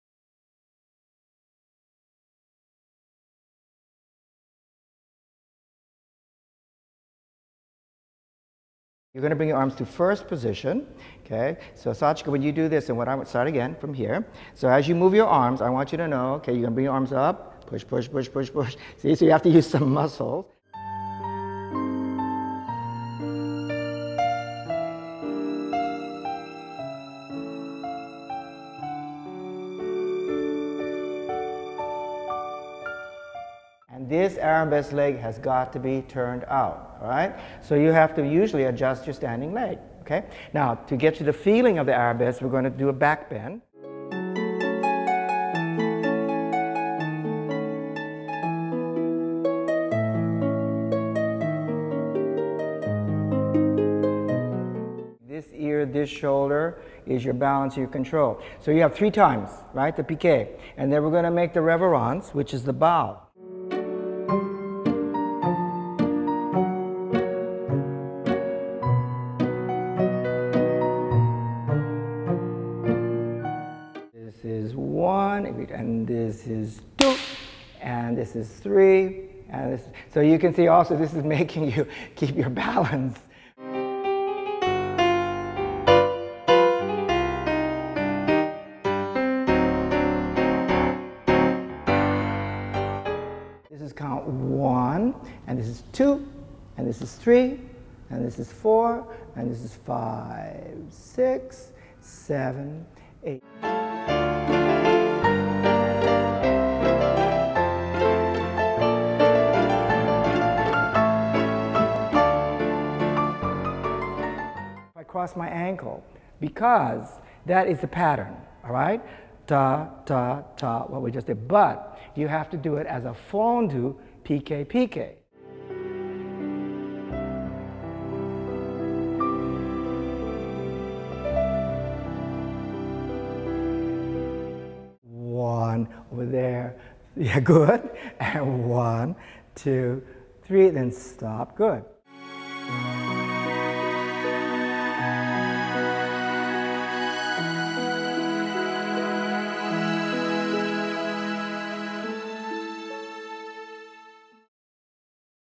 Basic Ballet Movement Skills Lesson 2
Finis Jhung shows you—count by count— how to shape and move your arms and hands with amplified grace and elegance; how to find and display your arabesque and balance the piqué pose; how to work the feet and balance in coupé; and how to waltz in various directions and positions. Each exercise has two parts—instruction and demonstration— and is shown moving to both right and left by Sachiko Kanai. Bonus sections include outtakes and an interview with Sachiko.